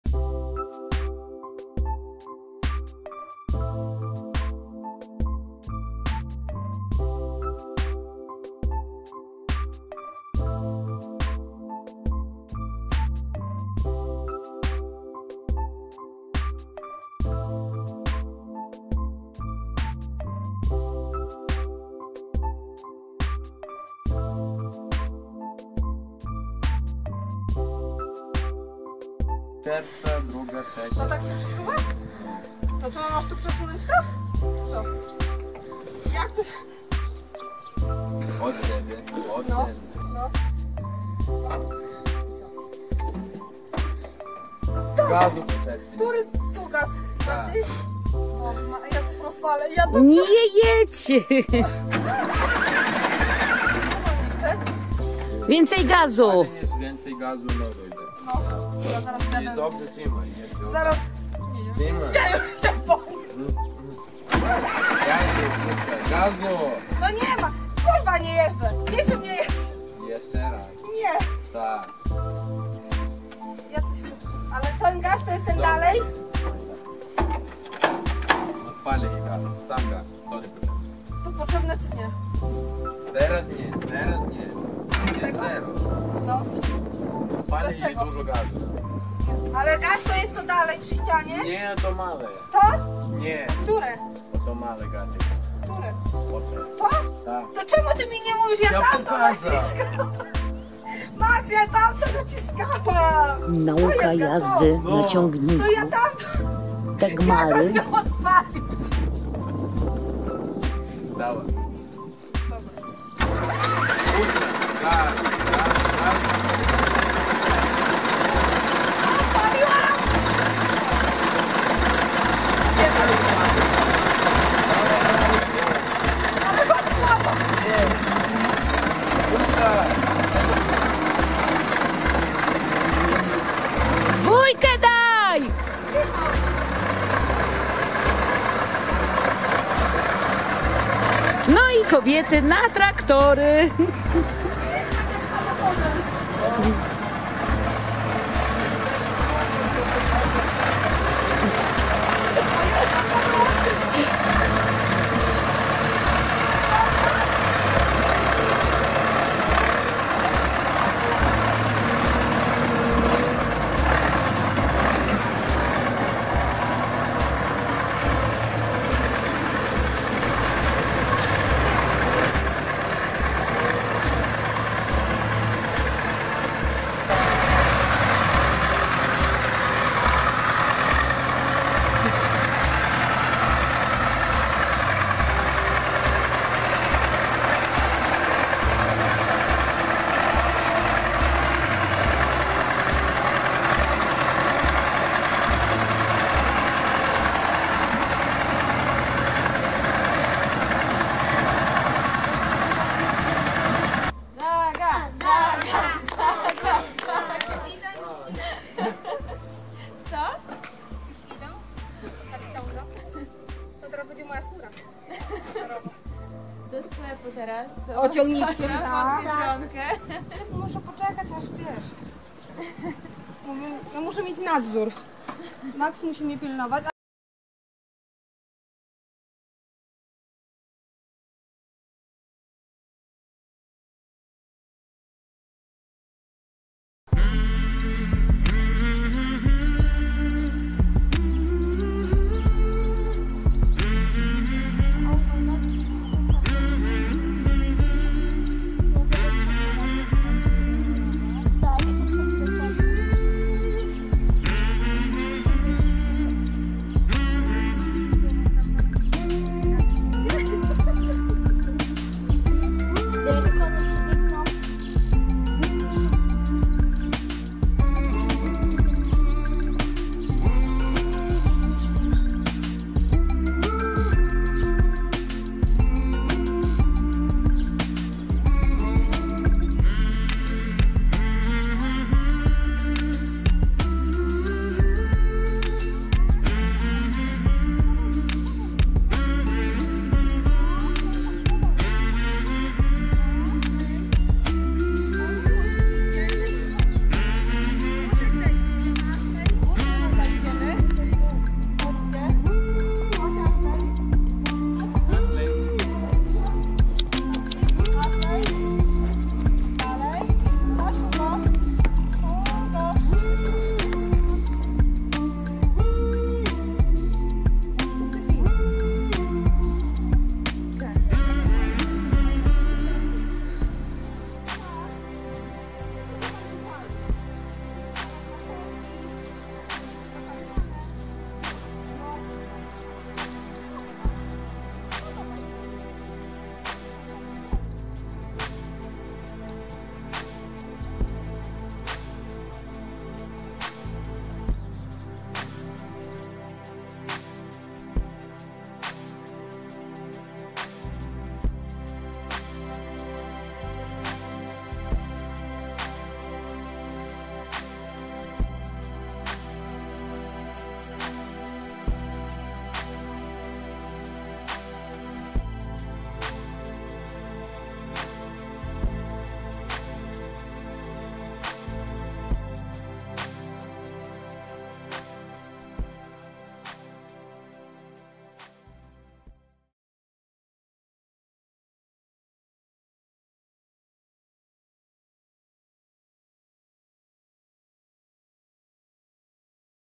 „Kobiety na traktory"
Dziś 1 maja tz. Święto Pracy.
W Tarze praca nigdy się nie kończy...
Przygotowaliśmy dla Was film „Kobiety na traktory"
Życzymy miłego oglądania i uśmiechu.